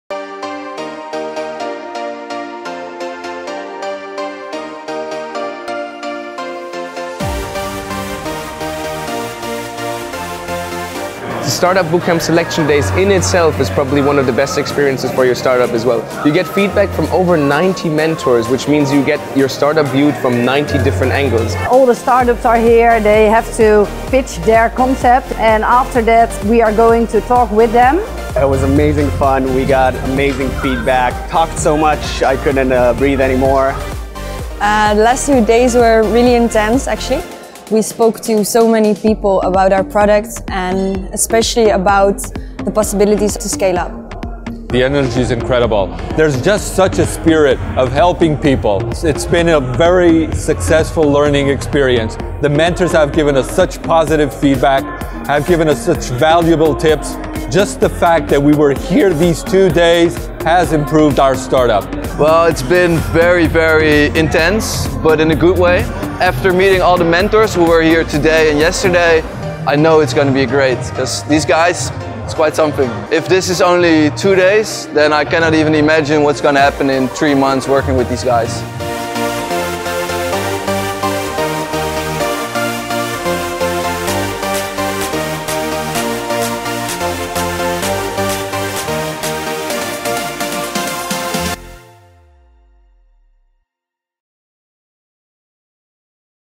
Startupbootcamp Smart City & Living 2017 Selection Days
Selection Days of our 2017 Smart City & Living program took place on the 21st and the 22nd of March in Amsterdam. Startups spent the 2 intense and exciting days pitching and convincing our mentors and partners of their disruptive powers.

Which 11 teams were selected to join the program and accelerate their businesses? To find out, read our blog post